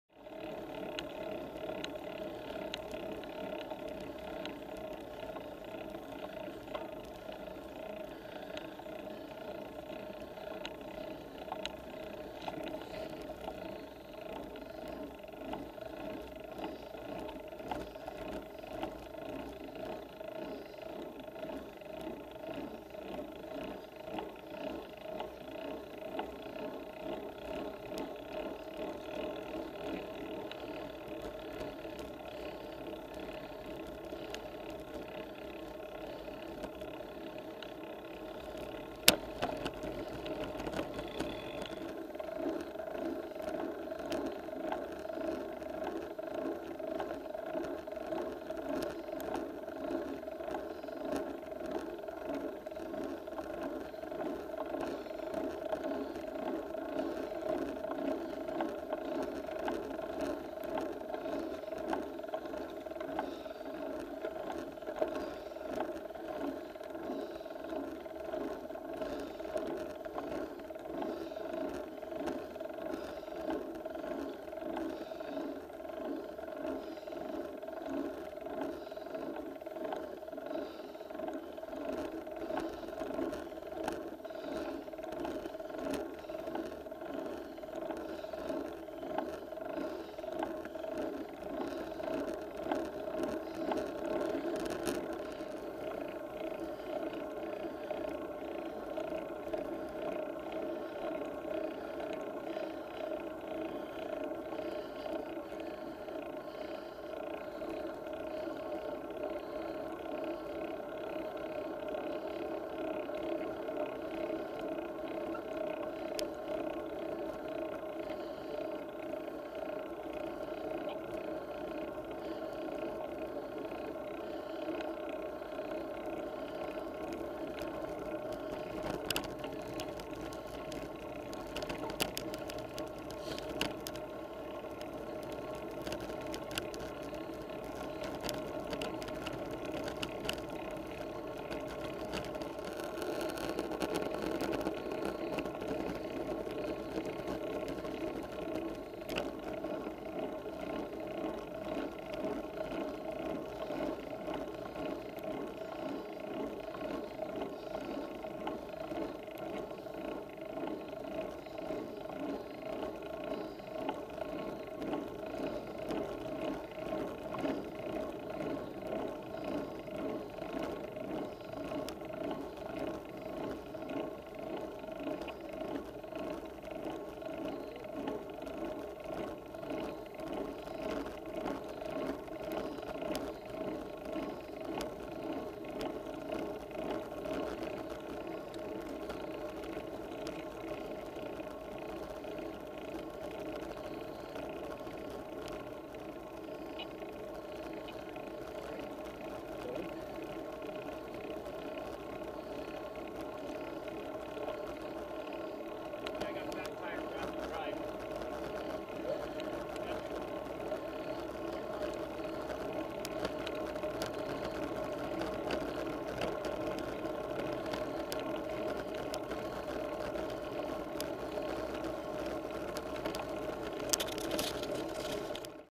Mt Charlie
First climb on Santa Cruz Mountain Challenge is Mt. Charlie.  They call it a warm up.